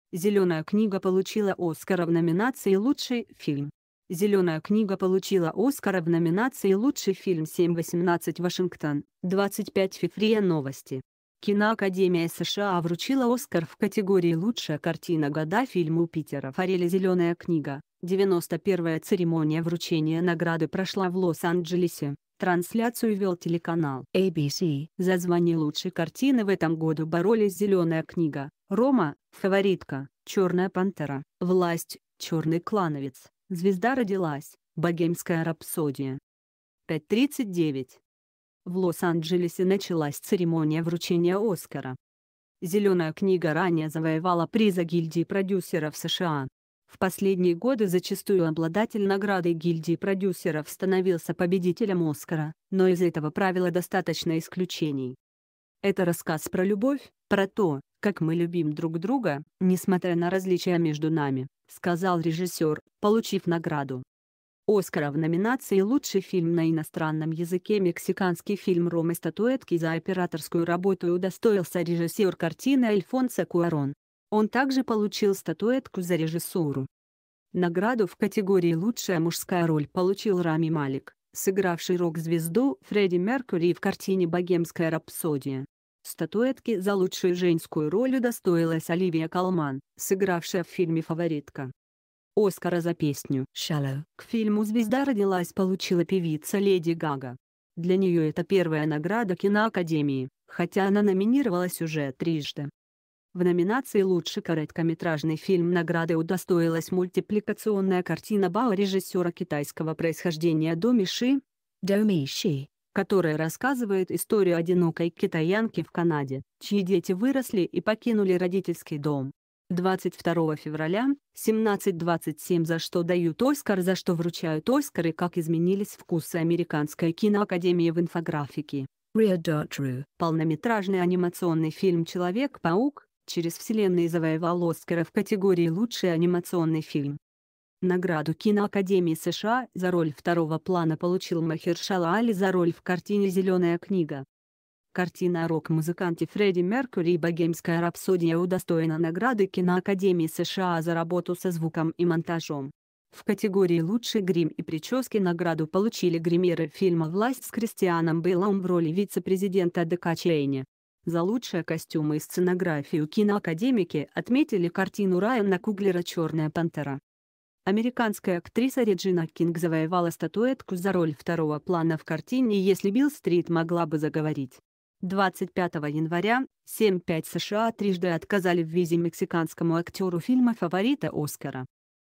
Зеленая книга получила Оскара в номинации лучший фильм
Зеленая книга получила Оскара в номинации лучший фильм Зеленая книга получила Оскара в номинации лучший фильм 07 18 ВАШИНГТОН, 25 фев РИА Новости . Киноакадемия США вручила Оскар в категории лучшая картина года фильму Питера Фаррелли Зеленая книга , 91-я церемония вручения награды прошла в Лос-Анджелесе, трансляцию вел телеканал ABC . За звание лучшей картины в этом году боролись Зеленая книга , Рома , Фаворитка , Черная пантера , Власть , Черный клановец , Звезда родилась , Богемс